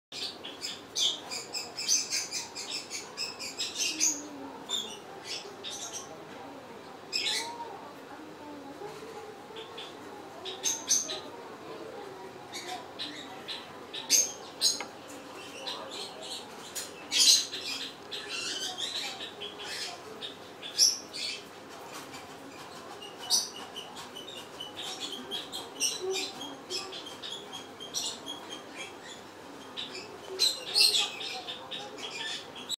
逆さま寝相のハムスター Hamster of an upside-down sleeping posture
にょるっとはみ出たハムスター。柔軟な身体を持った柔らかいハムスターです。

It is a soft hamster with the flexible body. Animal_Movie